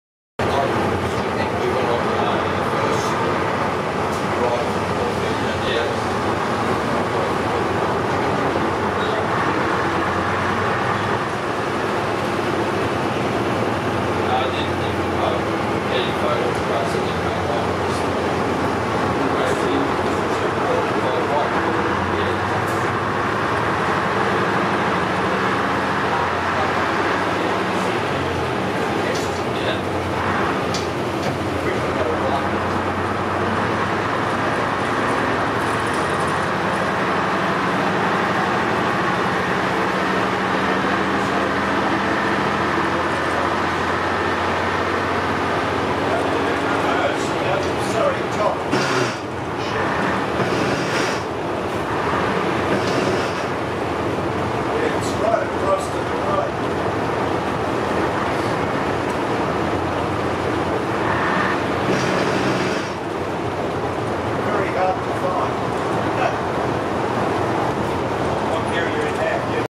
On board a 1948 White Motor Bus model 798 - 12 as it traveled to Yarra Glen Victoria.
Saturday 12th November 2016: BCSV Yarra Glen Heritage Bus Convoy.

On board a 1948 White Motor Bus model 798 -12 as it traveled from Mount Waverley to Yarra Glen Racecourse in Victoria Australia.  This bus used to operated under the lever Coaches livery. The bus was owned by Quodling Bros Queanbeyen NSW.